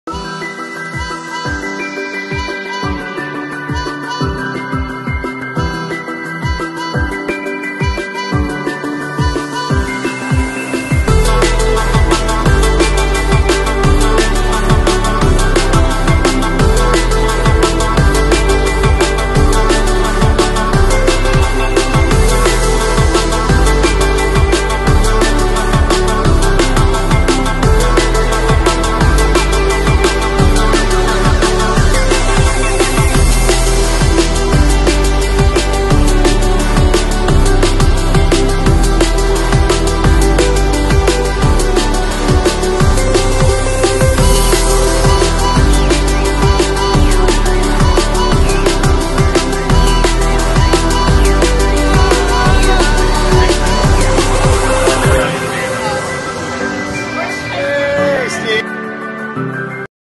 Deadman’s reef, Crescent Bay, Laguna Beach, California 2020
Just a fun dive on Deadman’s reef in Crescent Bay, Laguna Beach, California.